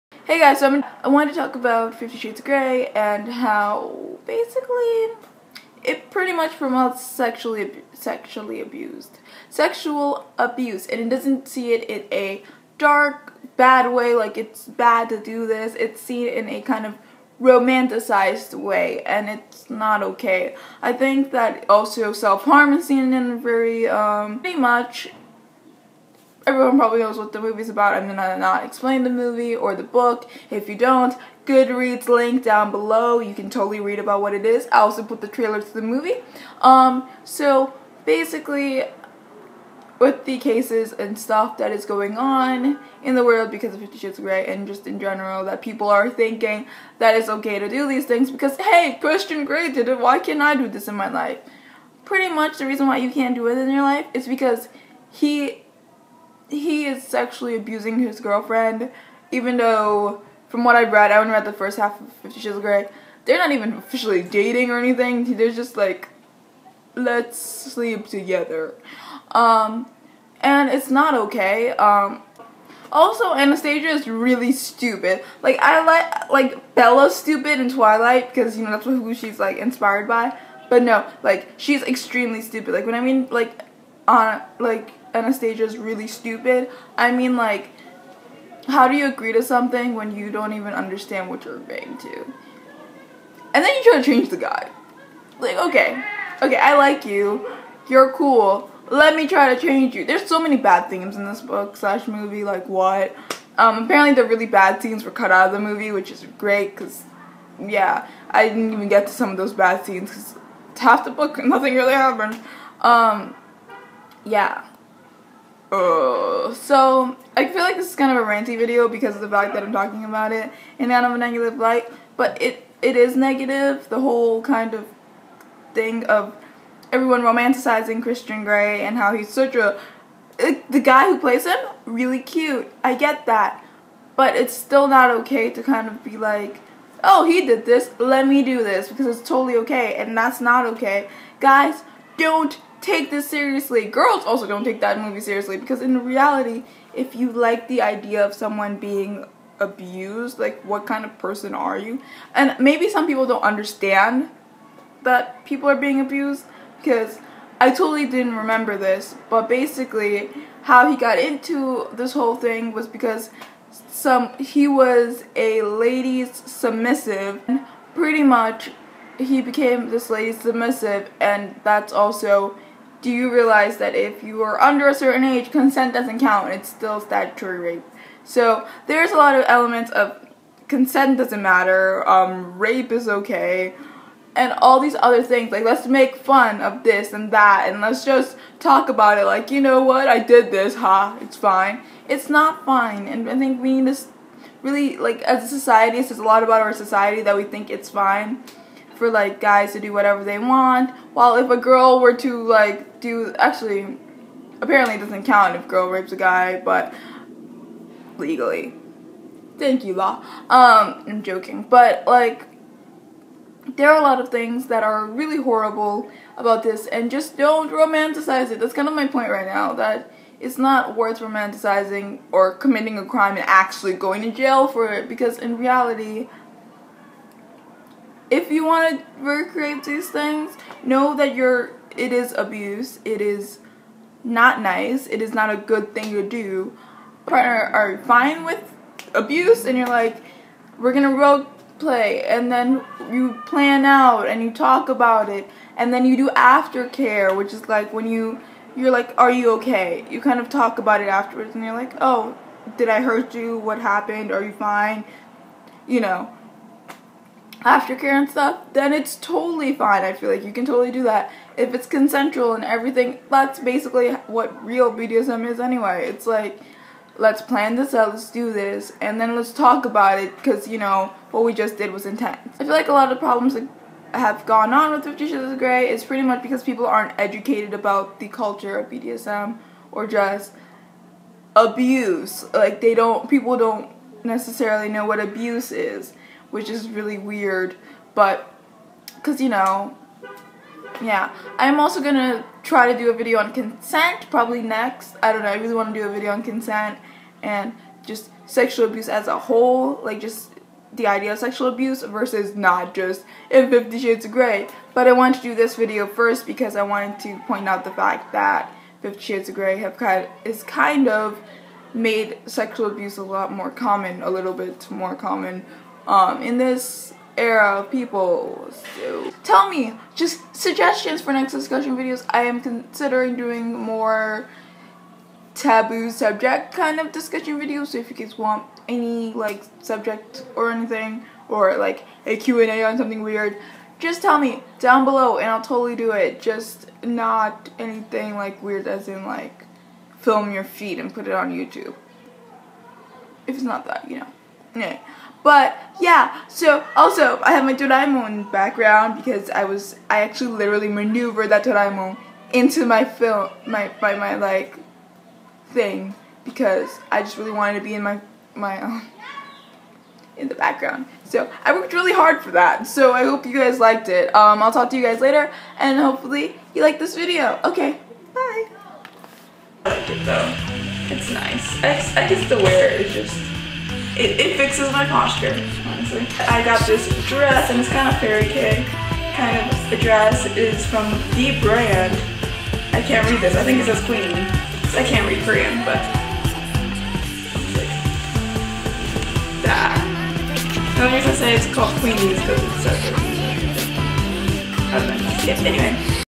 Lets Not Romanticize Sexual Abuse (50 shades of Grey)
Goodreads Link (50 Shades of Grey)
Movie Trailer